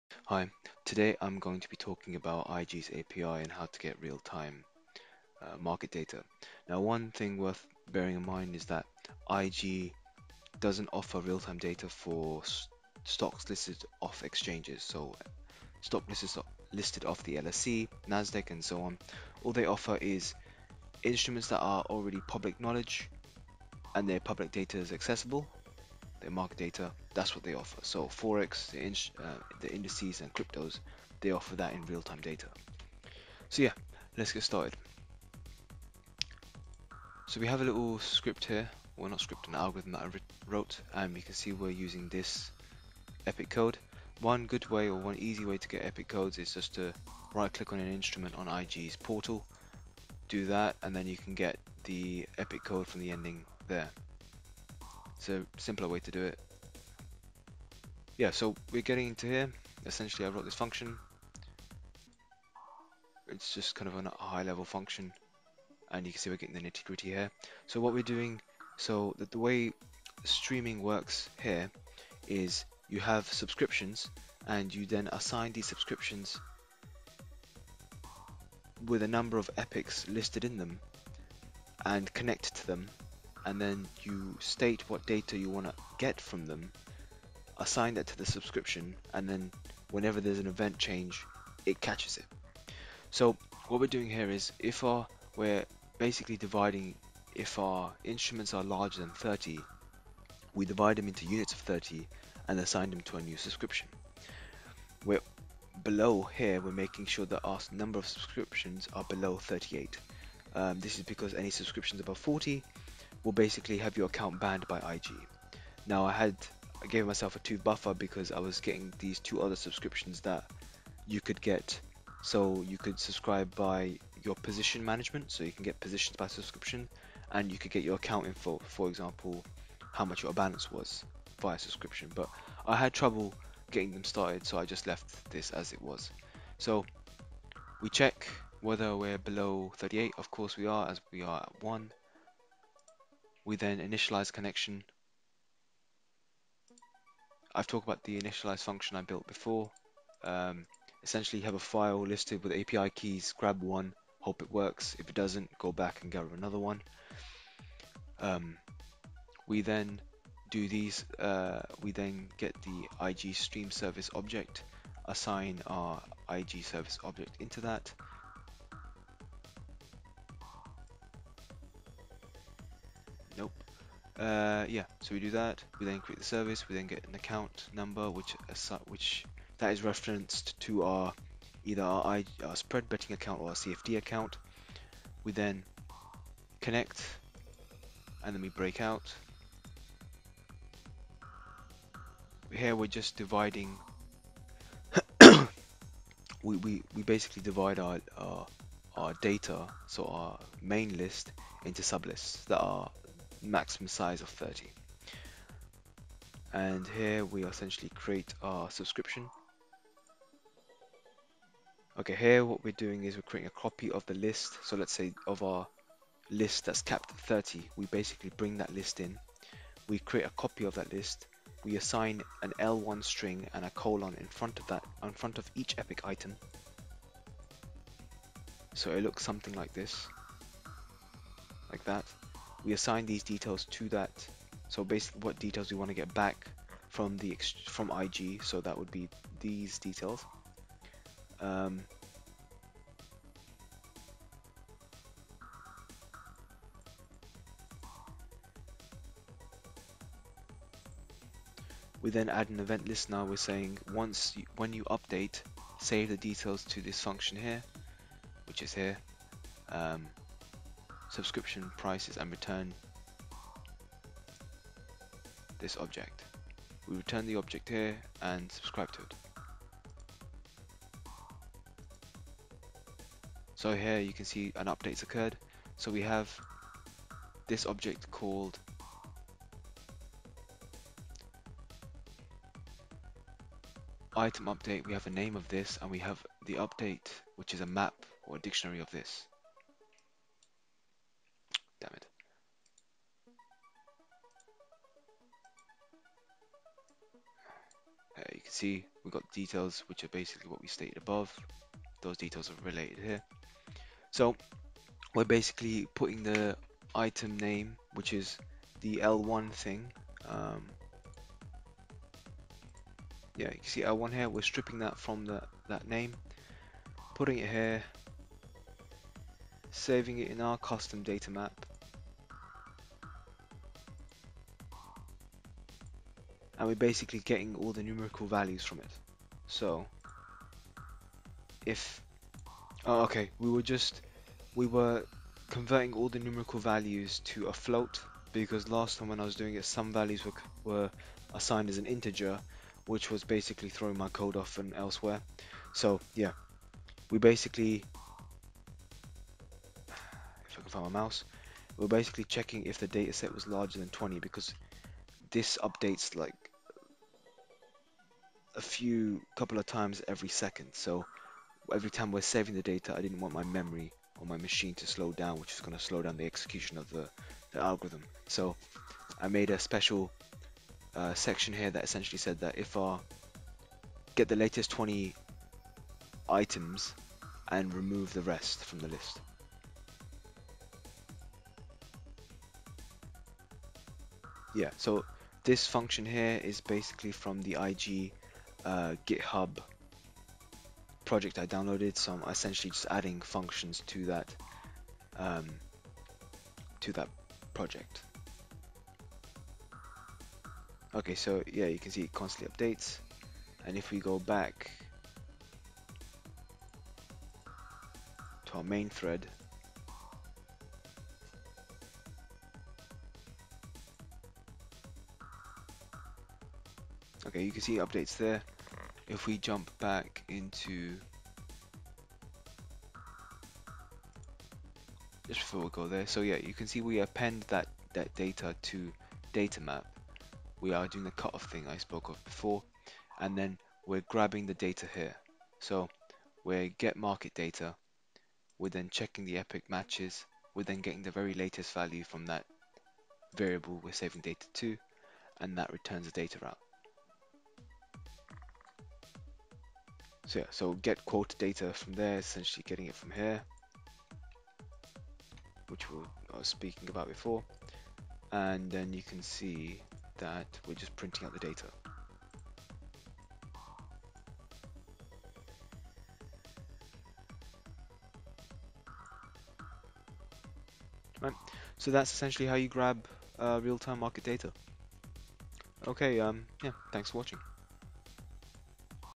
IG Trading Python API - Getting Realtime Data
Thanks. Getting Realtime Data via IG Trading API - using Streamers.

Realtime data retrieval Code is up on github watch vid to find out more: